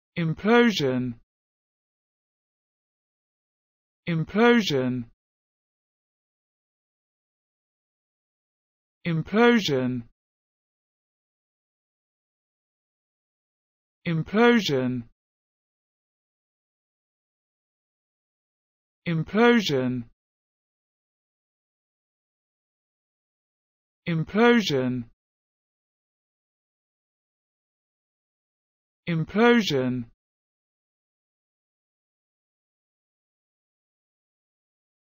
the correct pronunciation of innerly in English.
This video shows you how to pronunciation innerly in English.